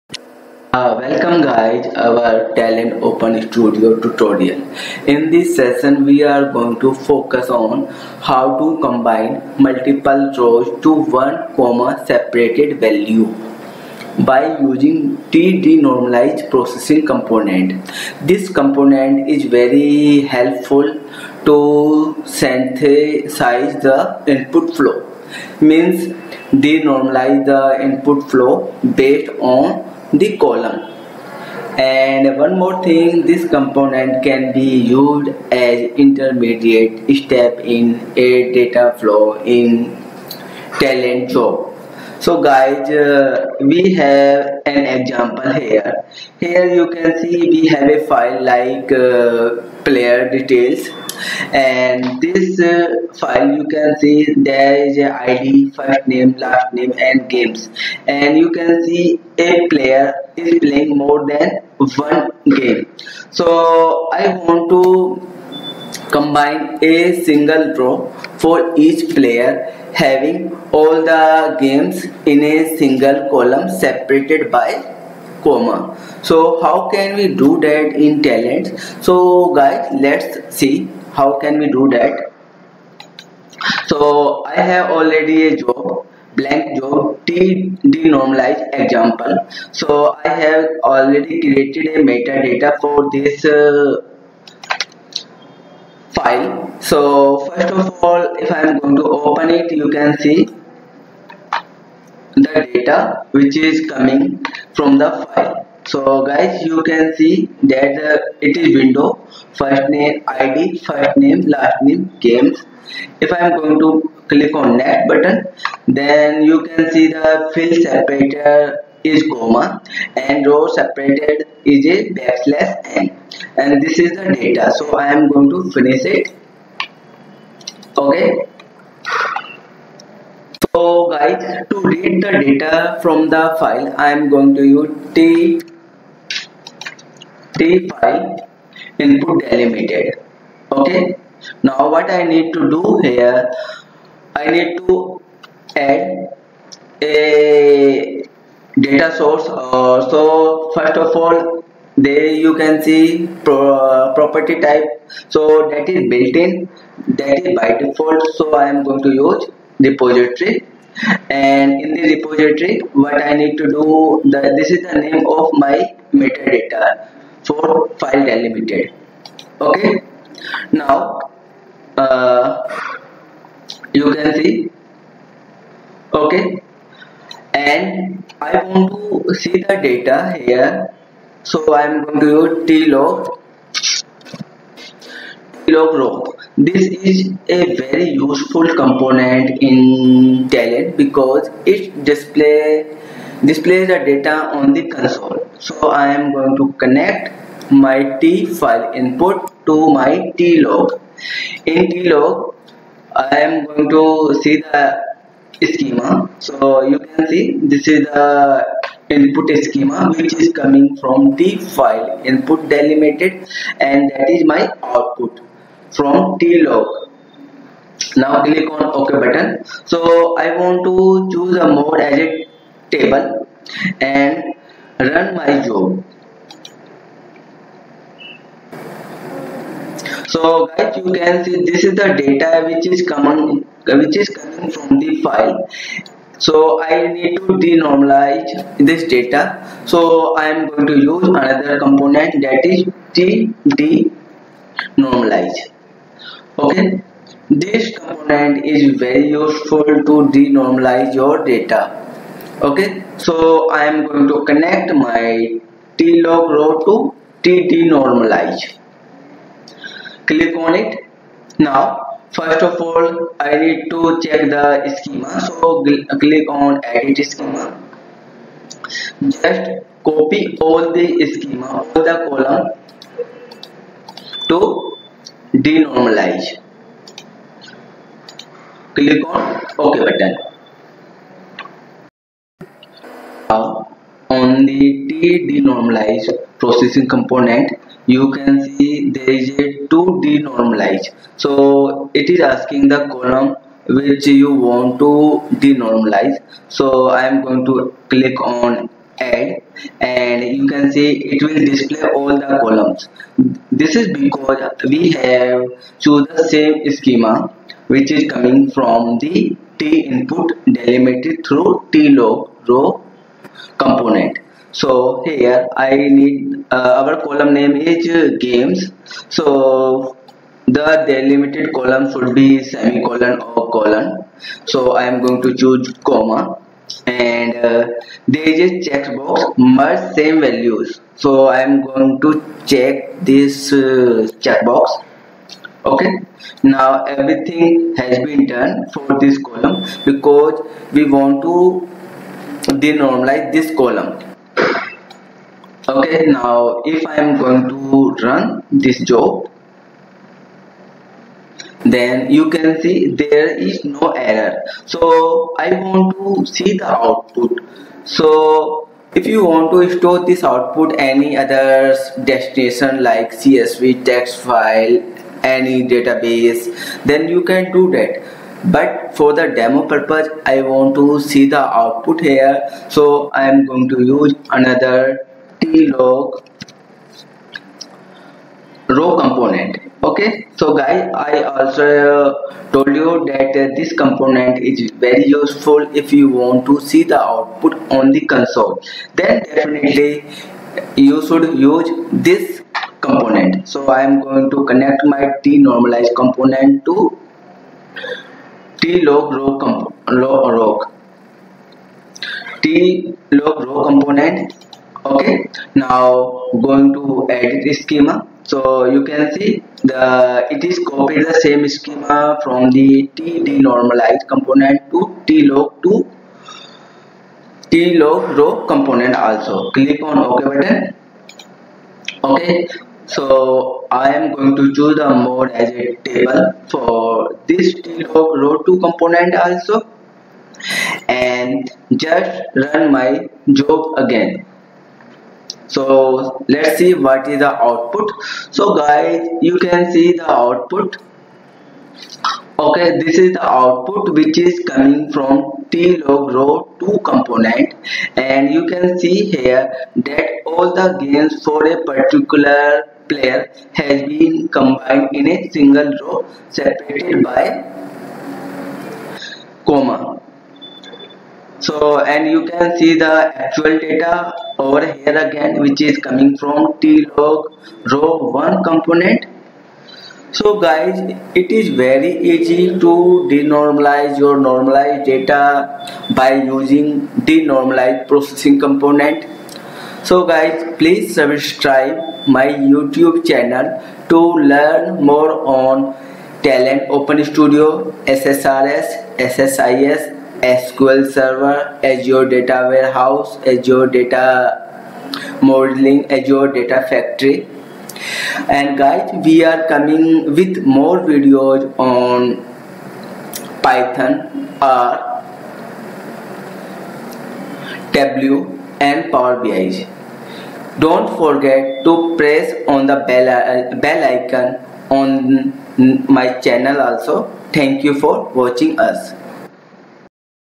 Talend ETL - Combine multiple rows to one comma-separated value
Here you will learn "How to combine multiple rows to one comma-separated value  by using tDenormalize processing component?" in Talend Open Studio.

tDenormalize processing component helps synthesize the input flow means denormalizes the input flow based on one column. This component can be used as intermediate step in a data flow.

This component is reverse of tNormalize processing component. In tNormalize it split records to multiple records .Here in tDenormalize it combine multiple records to single records.

We are using player data and every player plays multiple games and all the games for that player is stored in Games column.
We want to combine multiple rows from games column to one comma-separated value for each player.